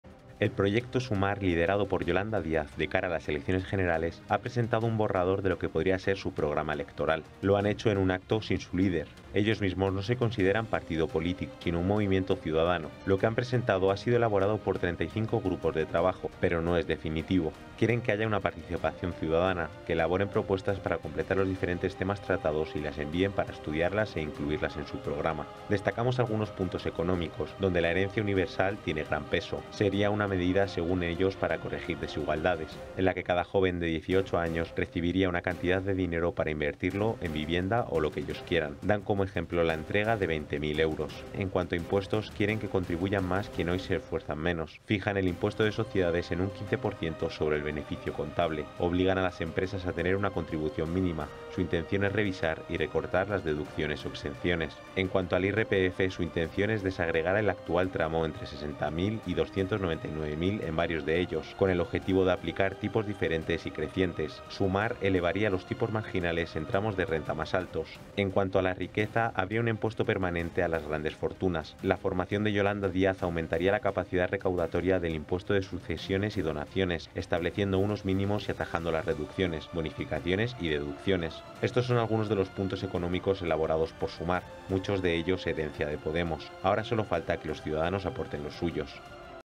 Las principales propuestas de Sumar: más impuestos, herencia universal y el 7% del PIB a Educación
Yolanda Díaz hace públicas las bases de su programa político y pide a sus votantes que hagan más propuestas. En materia económica, pide que existan más impuestos a las rentas más altas, que cuando los jóvenes cumplan 18 años reciban 20.000 euros o modificar los tramos de IRPF.